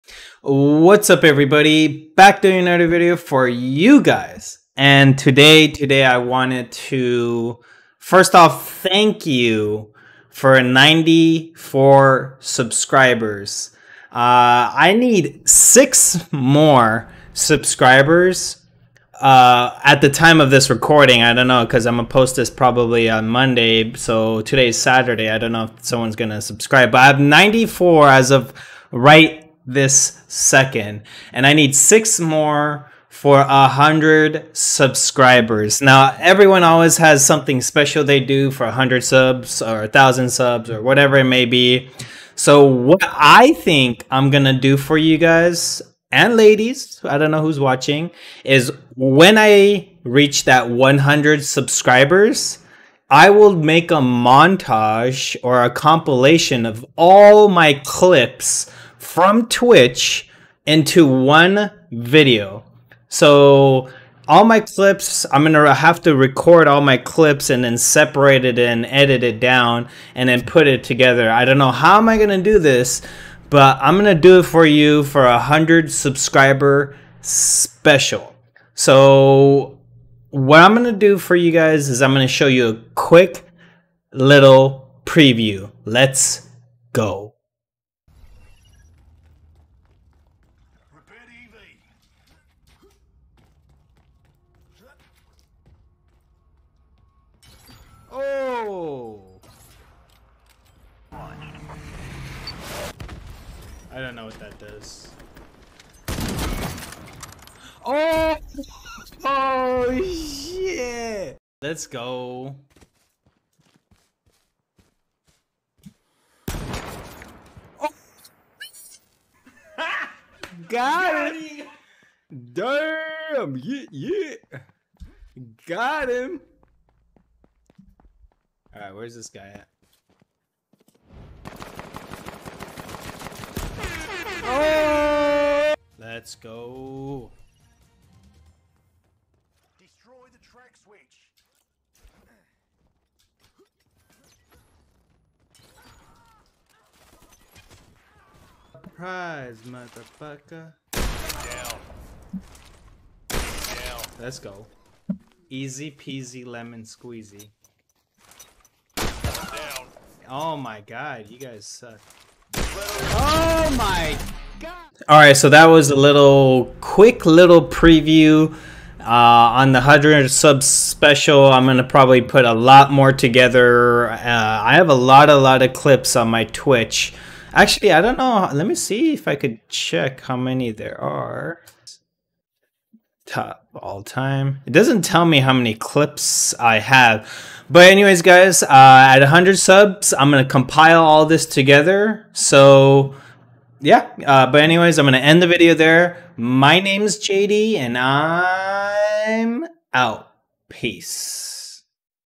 Only 6 More Subscribers For 100!!
Whats up everyone. Today I thank you for supporting me. For the 100 subscriber special i will be compiling all my clips or at least most of them into 1 video!! Let me know what you think!